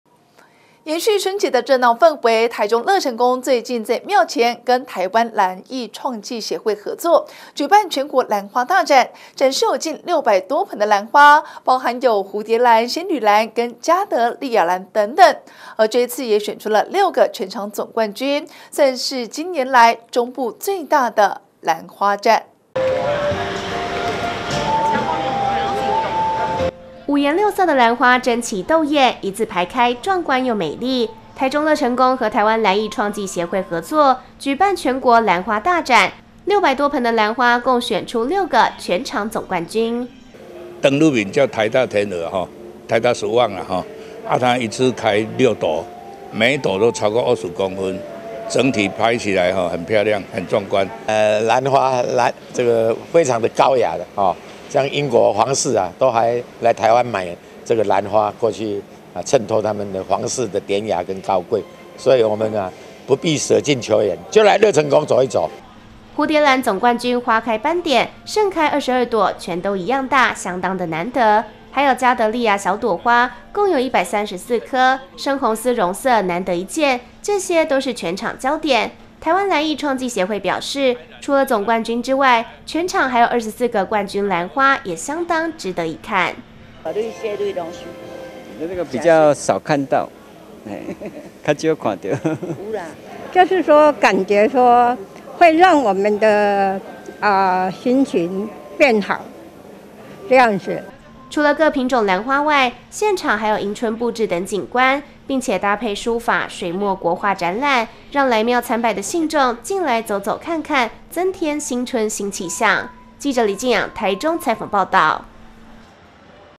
全國蘭花大展樂成宮登場 6百多盆花爭奇鬥艷 -大台中新聞
延續春節熱鬧氛圍，台中樂成宮最近就在廟前和台灣蘭藝協會合作，舉辦全國蘭花大展，展示近6百多盆的蘭花，包含蝴蝶蘭、仙履蘭和嘉德麗雅蘭等等，這次共選出6個全場總冠軍，算是今年來中部最大的蘭花展。【以上新聞可從哈TV APP收視】